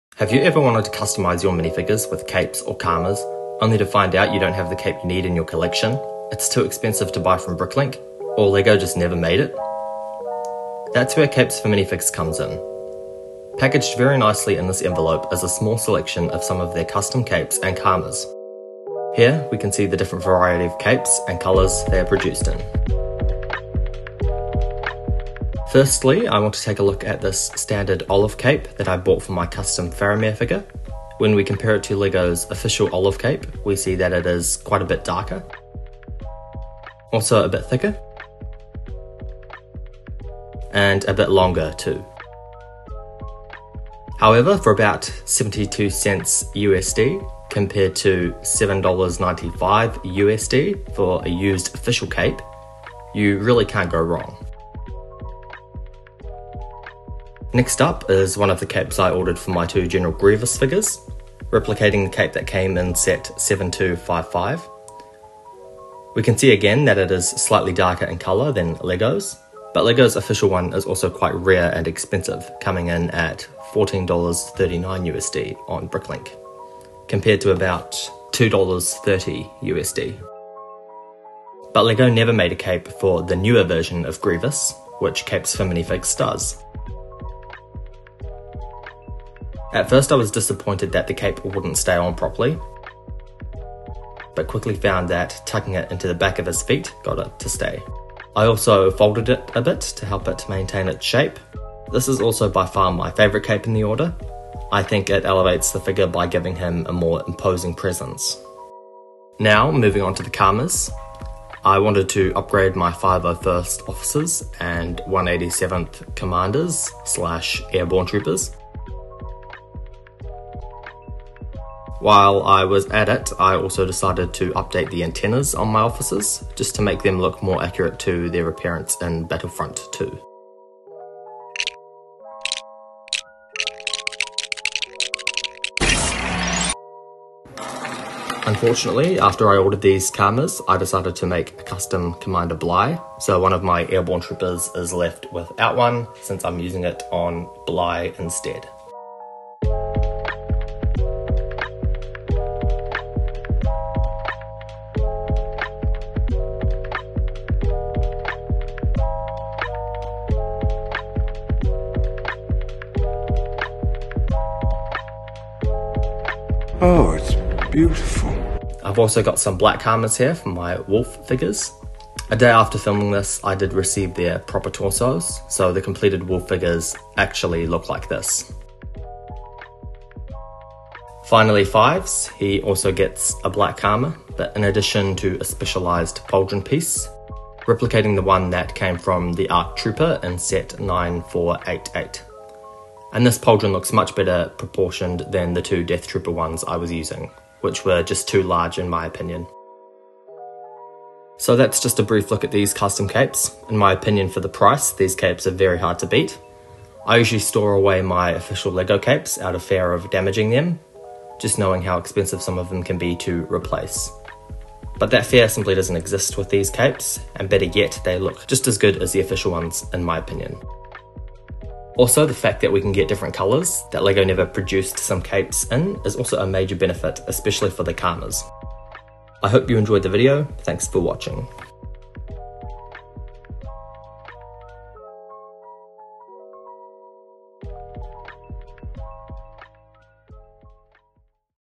The Best Lego Capes?
These Customs capes from Capes4Minifigs are a really nice alternative to Official Lego capes. Great for replacing old and damaged capes or customising minifigs at an affordable price.